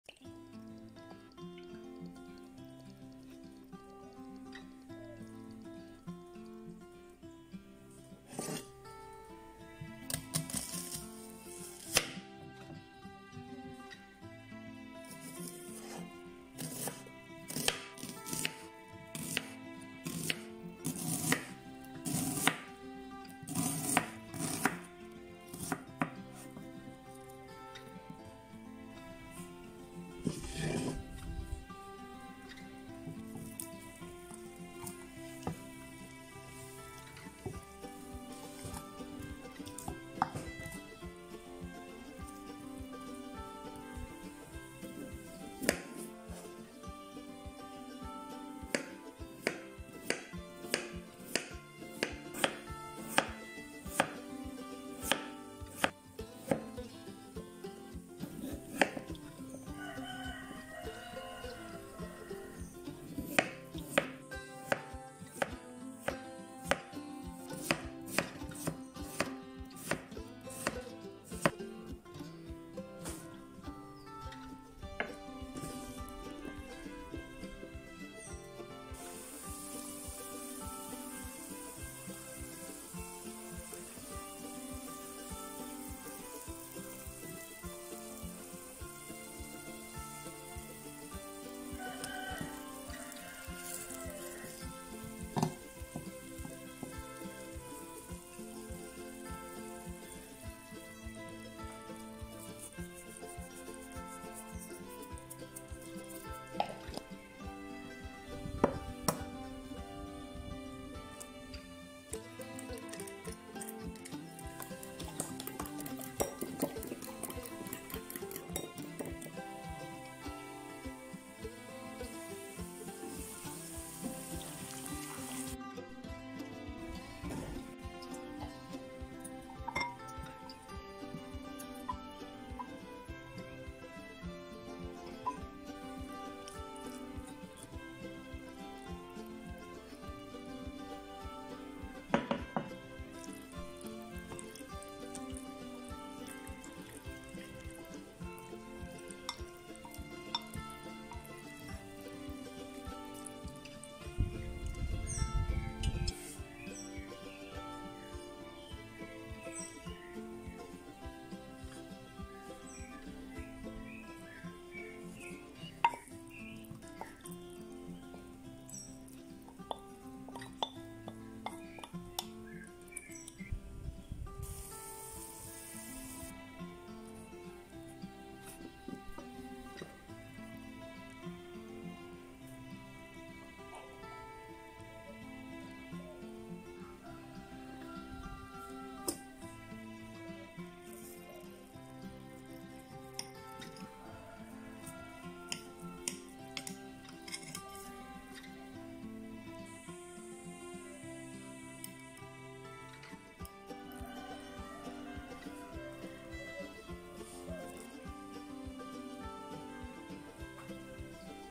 ¡Solo patatas y huevo! ¡ creiblemente simple y rápido receta.
Hola
Les traigo esta receta muy censilla y muy deliciosa.
Está deliciosa  espero les guste y se animen a preparar.
Cuéntame en los comentarios que te pareció.

Regalame un like
Comparte
Comenta

INGREDIENTES
Aceite
3 papas pequeñas
1/2 cebolla
1 huevo